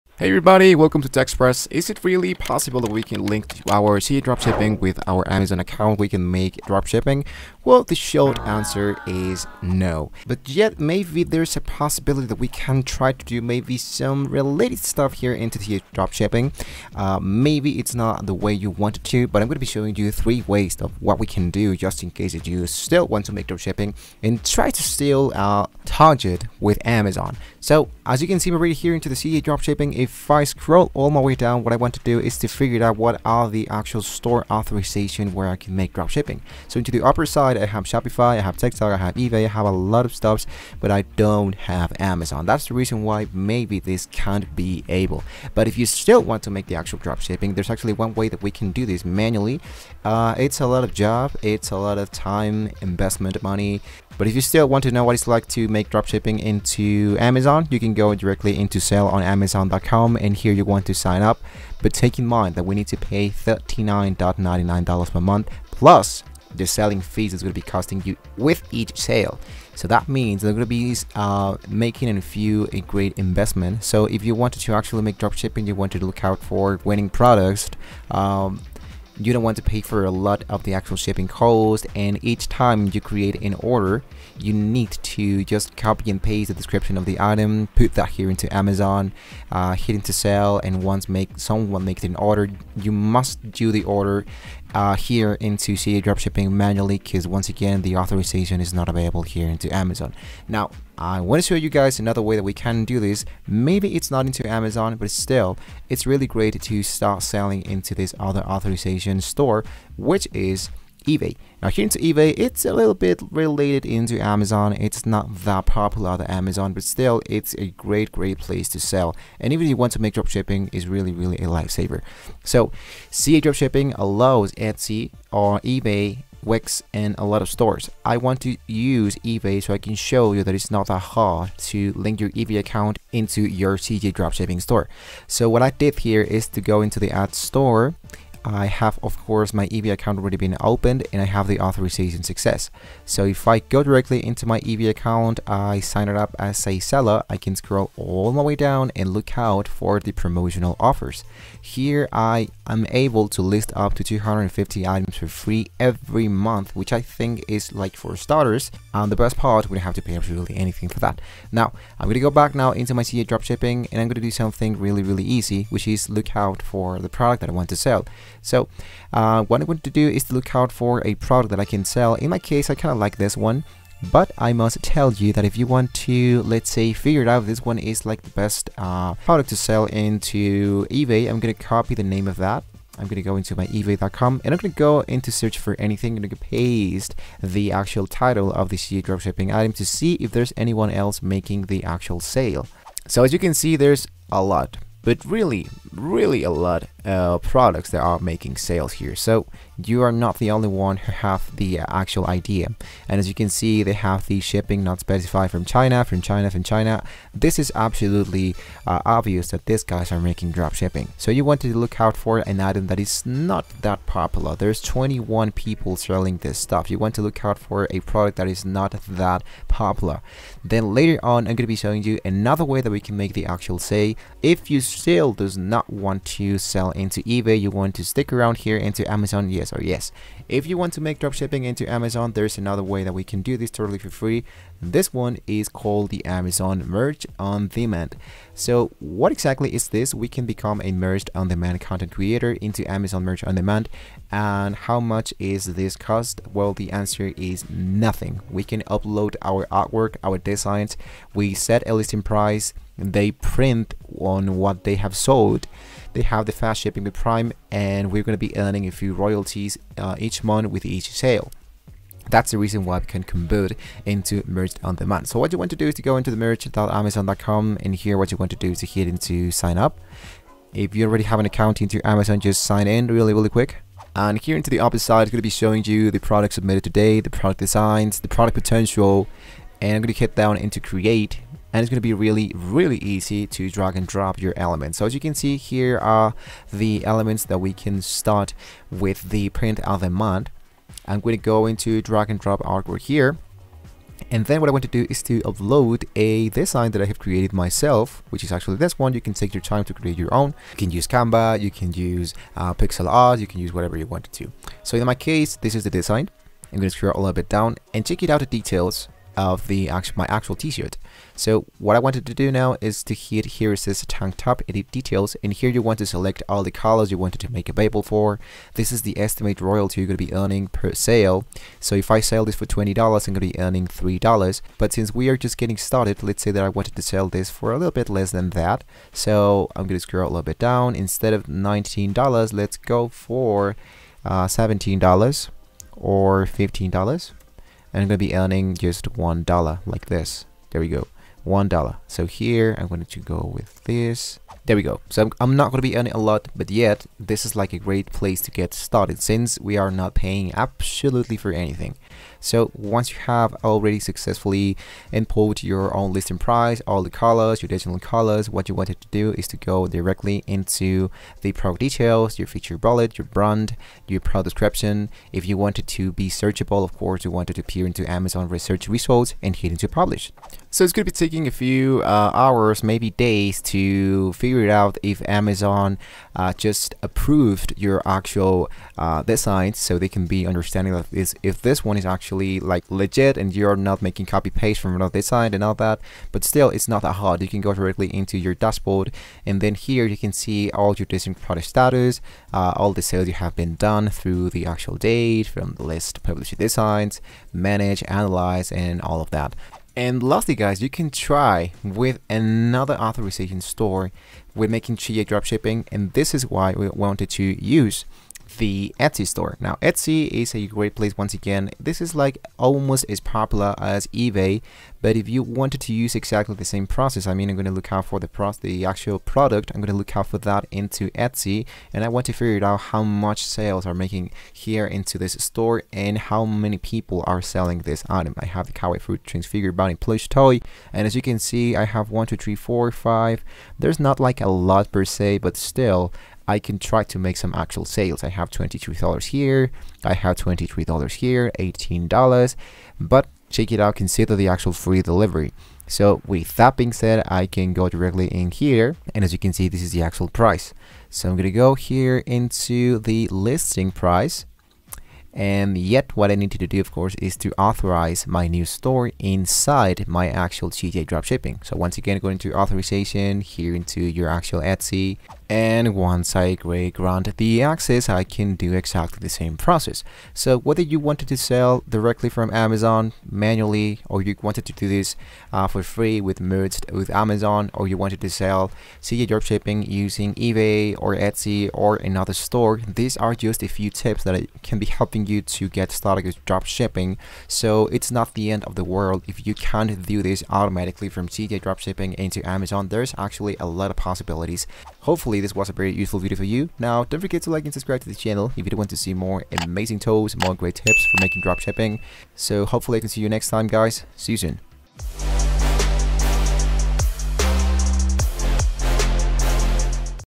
How to Link Amazon With Cj Dropshipping (2026) Amazon Dropshipping With Cj Dropshipping
Linking Amazon with CJ Dropshipping is indeed possible, and it can be done in a few simple steps. First, you need to create an account with CJ Dropshipping, which is a platform that allows you to source products from China and have them shipped directly to your customers. Once you have an account, you can then link it with your Amazon account by using the Amazon Marketplace Web Service (MWS) API.

This will allow you to easily list and sell CJ Dropshipping products on Amazon, and have them fulfilled by CJ Dropshipping. With this integration, you can streamline your e-commerce operations and expand your product offerings, while also taking advantage of CJ Dropshipping's competitive pricing and fast shipping times. ‎